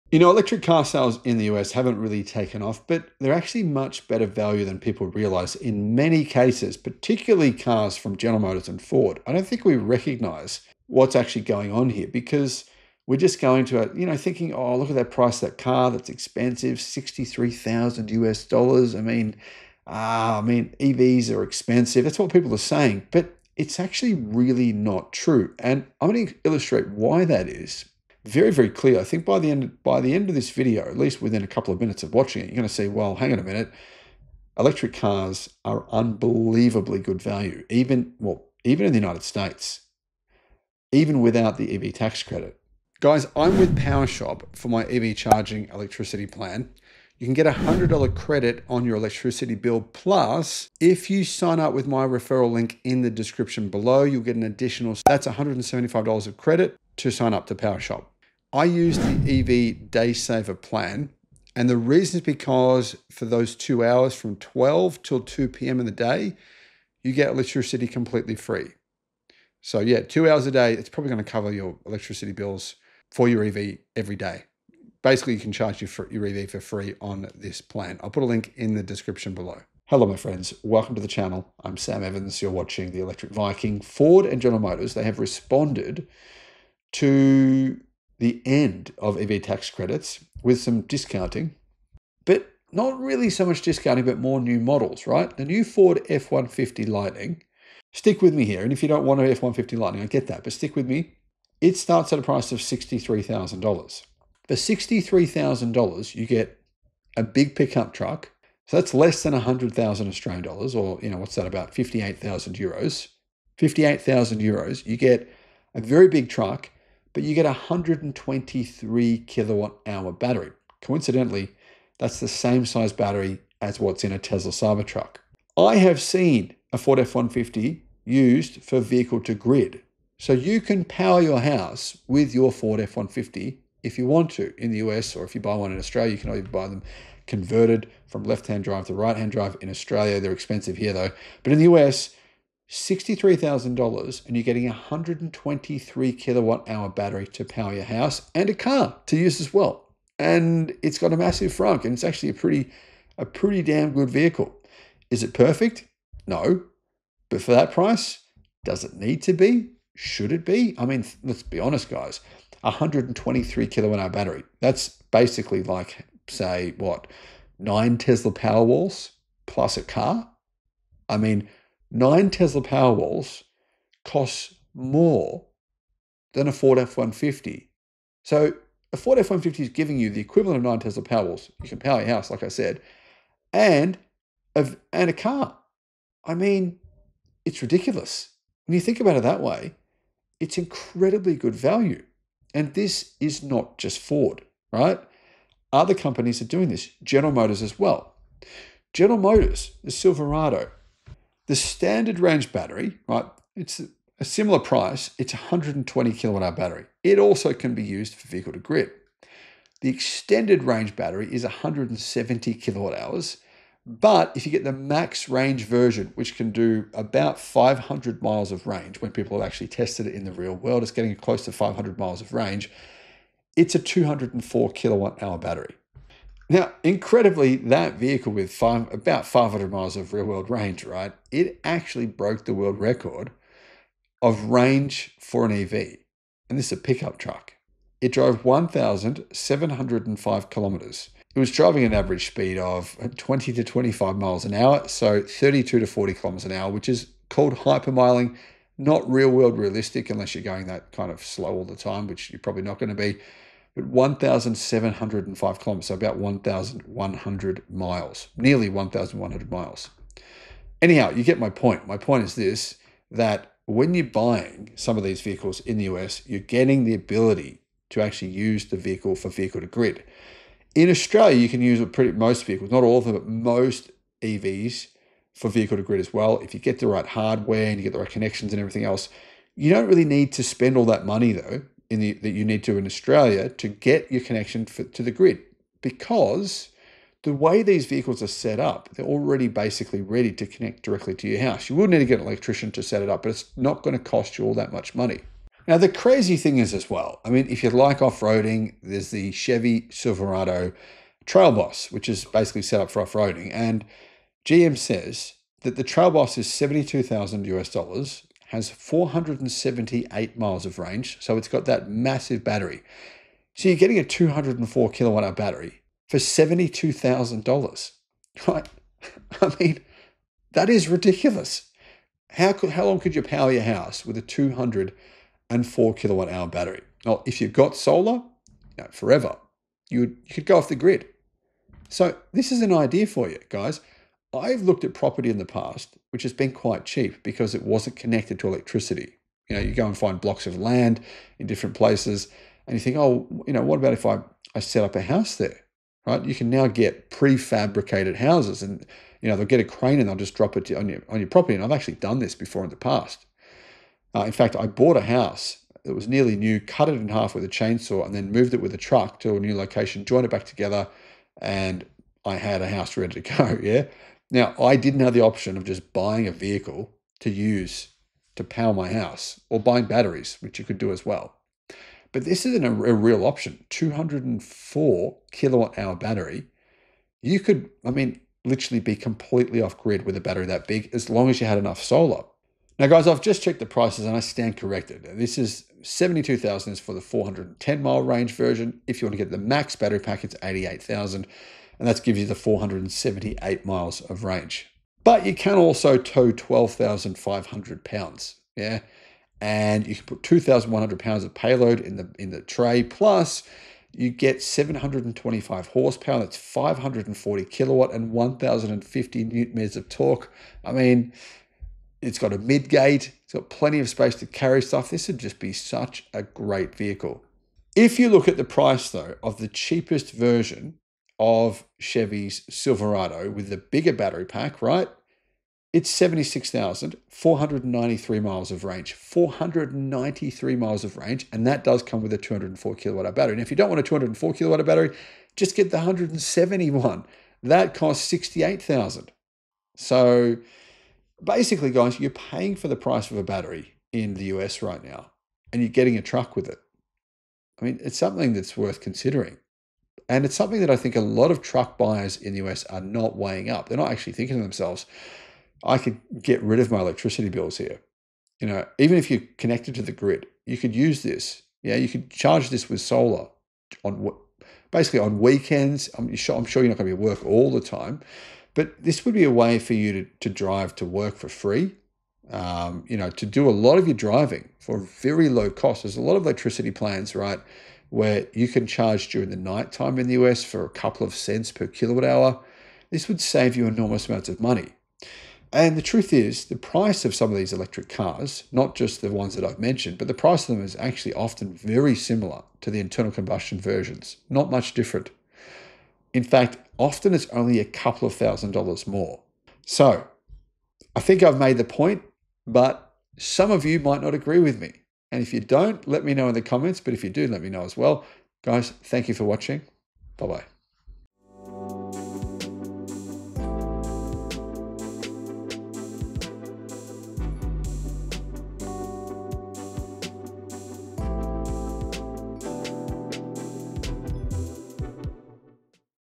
These 2 American Made Electric Cars Could Power Your House Forever
Two American-made electric trucks — the Ford F-150 Lightning and the Chevy Silverado EV RST — are redefining what it means to own a vehicle. Beyond towing power and instant torque, these trucks can literally power your home for days, thanks to bidirectional charging systems that turn them into giant mobile batteries on wheels.

In Italy you can buy a 200 mile range EV for less than the price of a Bicycle